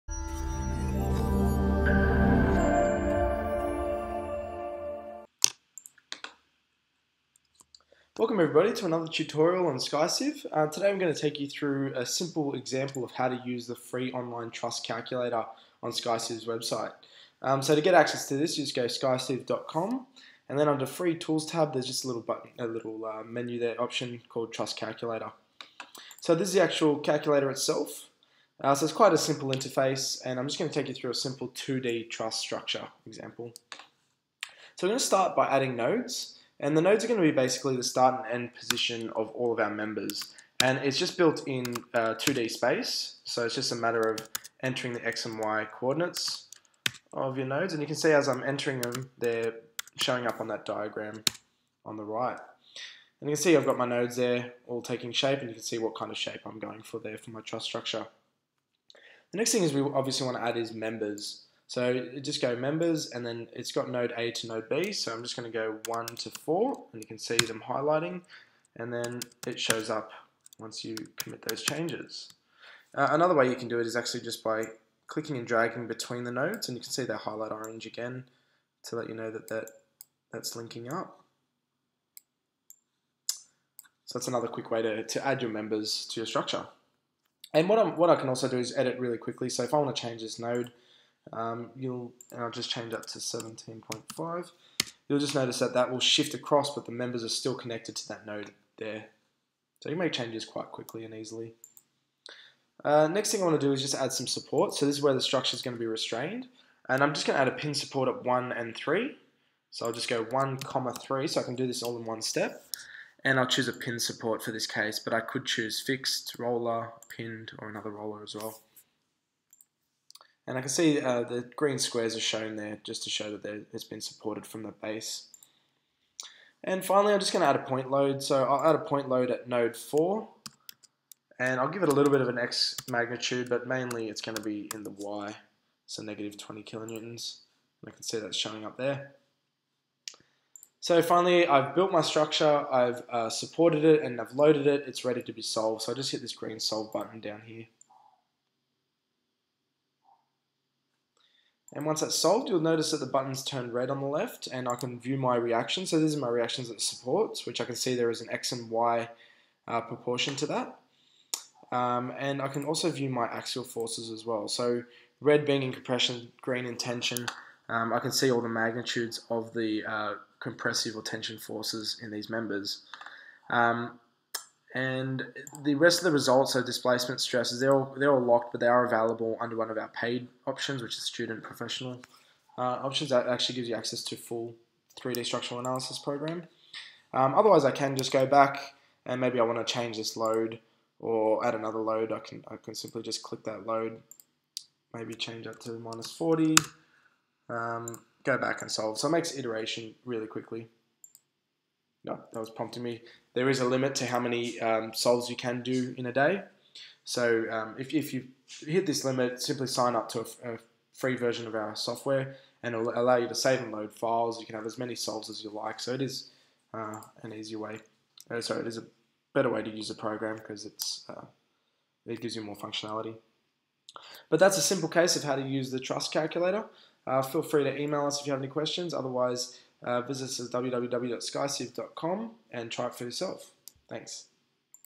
Free Online Truss Calculator Tutorial - SkyCiv Engineering
A short tutorial on how to build and solve a truss structure using the SkyCiv free online truss calculator.